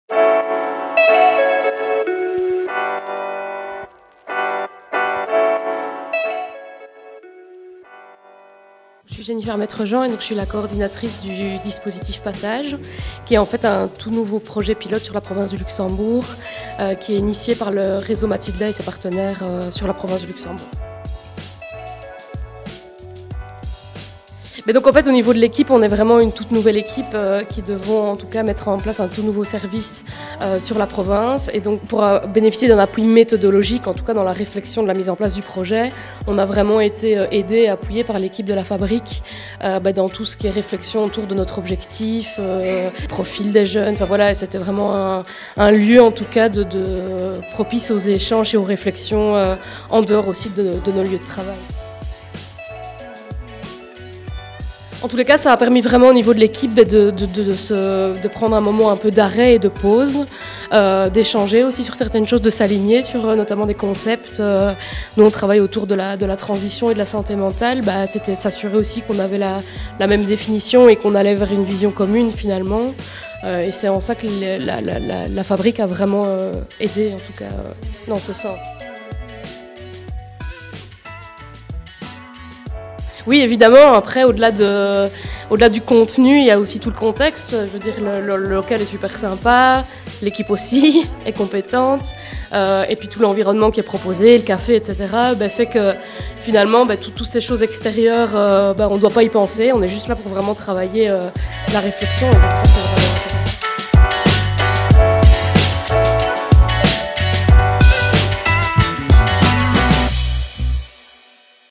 La Fabrique à projets, ils vous en parlent ! Jennifer Maîtrejean
Pour mettre en place un projet ensemble et qu’il puisse aboutir, il est indispensable que les acteurs parlent le même langage et aient une vision commune de leurs objectifs.
Les techniques d’animation en intelligence collective permettent d’organiser les pensées, de stimuler la créativité, d’aligner des visions, … en permettant à chaque participant de s’exprimer et s’engager dans les différentes étapes d’un projet.
C’est dans ce cadre que l’équipe du service « Pass-âge » a fait appel à La Fabrique.
Jennifer Maîtrejean, Coordinatrice du dispositif, vous présente ce que cela leur a apporté.

Les services proposés par la Fabrique à projets sont gratuits.